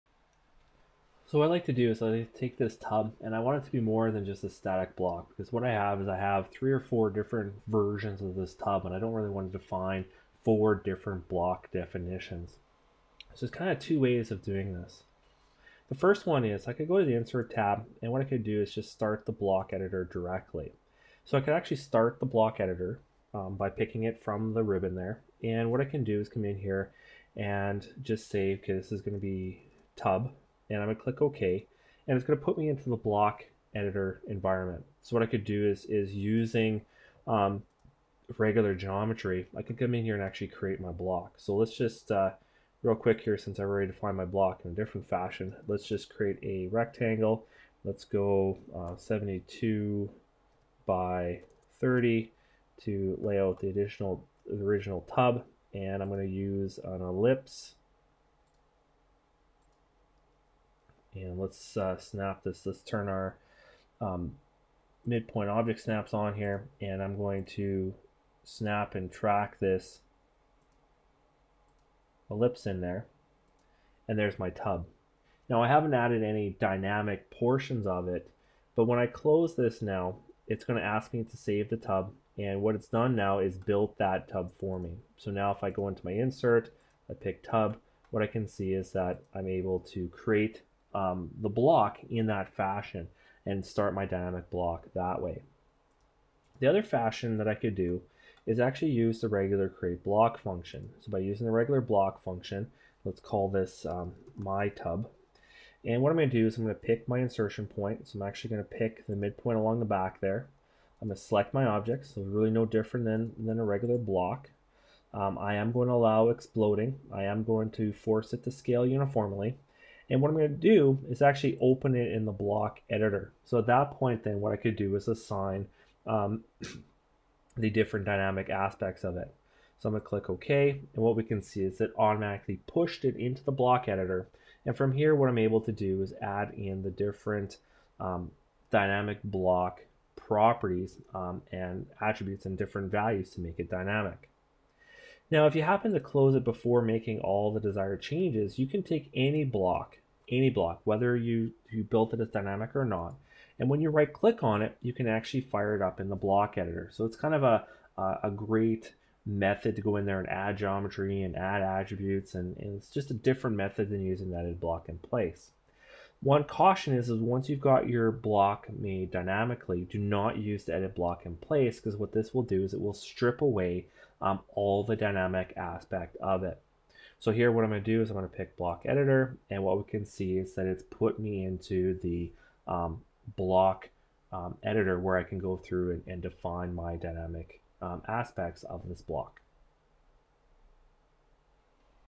Creating Dynamic Blocks with AutoCAD's Block Editor
The basics of starting Dynamic Blocks using the Block Editor within AutoCAD